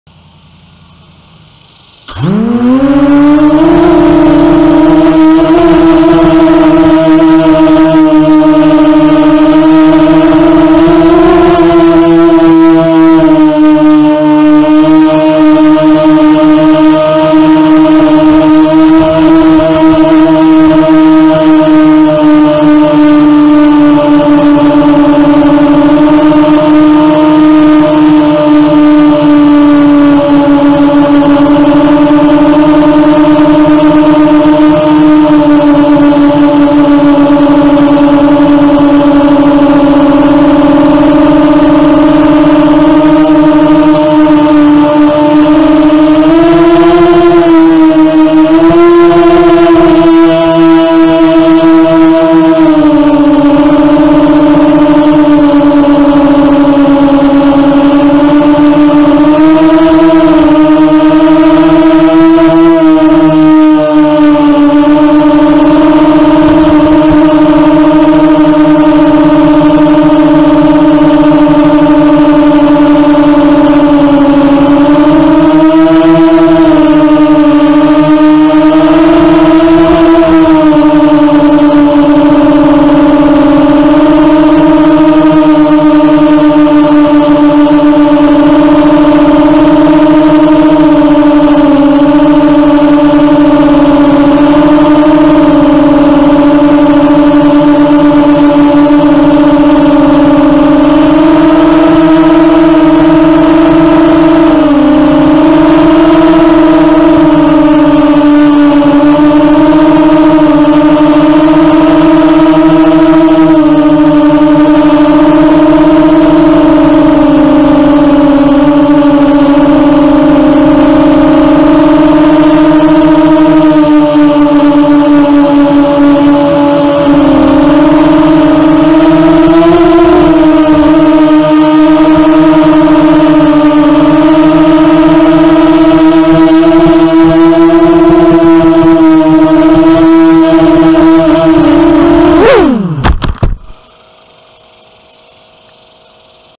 Epic 280 Tree laps 1
Went back to Suncreek Park today. Amazing day with a slight breeze so I figured I'd sit in the shade and see if any trees jumped out in front of me. I have to say I LOVE to switch to the bullnose props. So much easier to stay low. I was running with 6x4.5x3s but I think I will be staying with the bullnose for a while.

Epic 280 V1
2212 x 1800kv
30 amp esc
HQ 6x4.5 Bullnose
4s & 5s 2200mah zippy